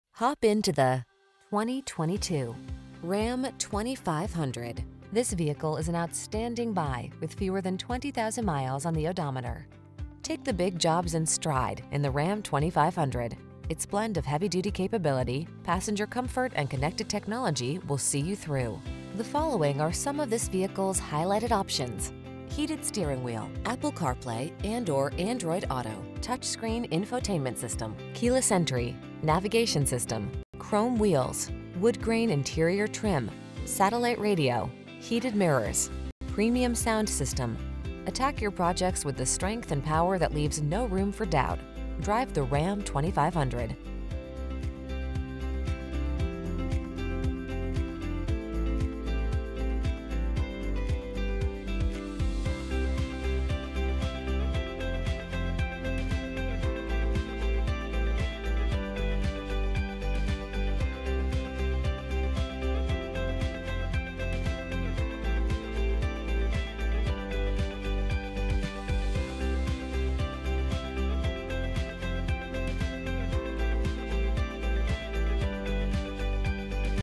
2022 Ram 2500 Limited GA Canton, Woodstock, Holly Springs, Cumming, Jasper
Pre-Owned 2022 Ram 2500 Limited available at Shottenkirk CDJR of Canton located in 200 Liberty Blvd, Canton, GA, 30114. Servicing the Canton, Woodstock, Holly Springs, Cumming, Jasper area.
Please mention the stock PRR1041 when you contact the dealership for this vehicle.

Hop into the  2022  Ram 2500.  Take the big jobs in stride in the Ram 2500. Its blend of heavy-duty capability, passenger comfort, and connected technology will see you through. The following are some of this vehicles highlighted options: Heated Steering Wheel
, Apple Carplay®/Android Auto®, Touchscreen Infotainment System, Keyless Entry, Navigation System, Chrome Wheels, Woodgrain Interior Trim, Satellite Radio, Heated Mirrors, Premium Sound System. Attack your projects with the strength and power that leaves no room for doubt. Drive the Ram 2500.